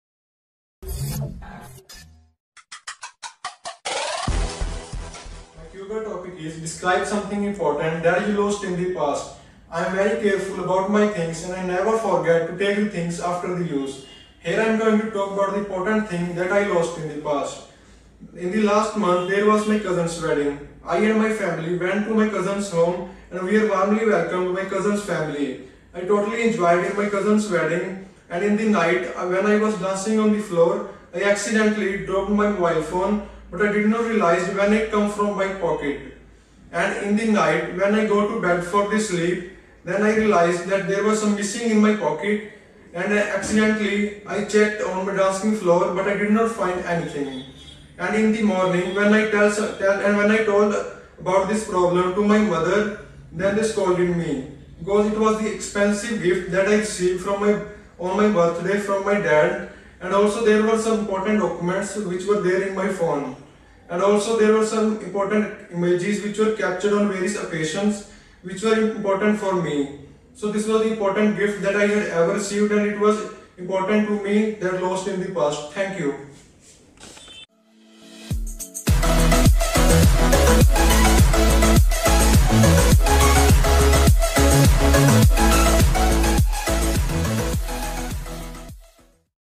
Describe something important that you lost in the past
Cue Card Sample answer, Describe something important that you lost in the past,  IELTS speaking,  ielts cue card, IELTS cue card explanation, IELTS speaking test,  IELTS interview